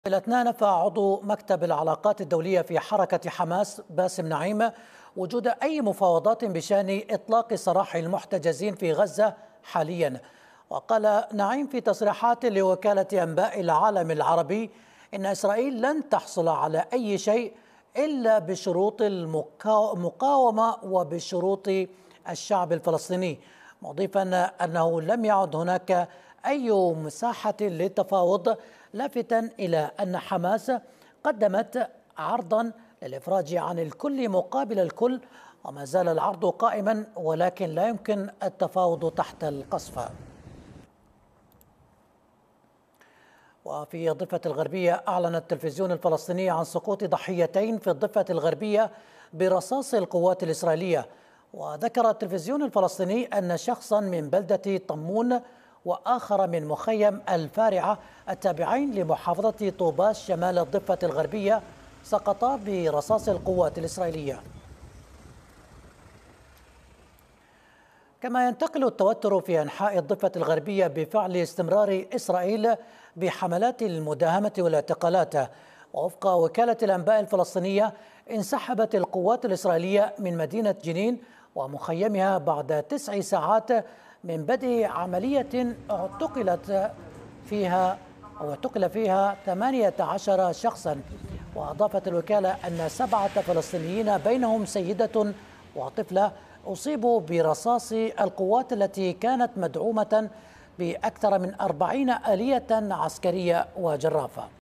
الفصائل في غزة توجه رسالة قوية لإسرائيل بشأن صفقة "الكل مقابل الكل" - أخبار الشرق
نفى باسم نعيم، عضو مكتب العلاقات الدولية بحركة "حماس"، وجود أي مفاوضات أو مباحثات بشأن إطلاق سراح المحتجزين في قطاع غزة في الوقت الحالي. وقال نعيم في تصريحات لوكالة أنباء العالم العربي "AWP" "إسرائيل لم تتعلم من درس الجولة الأولى، ففي 53 يوماً، دمر قطاع غزة وقتل الآلاف لعله يستطيع أن يحرر أحد أسراه، ولكنه فشل، ويريد أن يكرر التجربة". وشدد على أن "إسرائيل لن تحصل على أي شيء إلا بشروط المقاومة وبشروط الشعب الفلسطيني.. لا أعتقد أنه ما زال هناك أي مساحة أو هامش للتفاوض".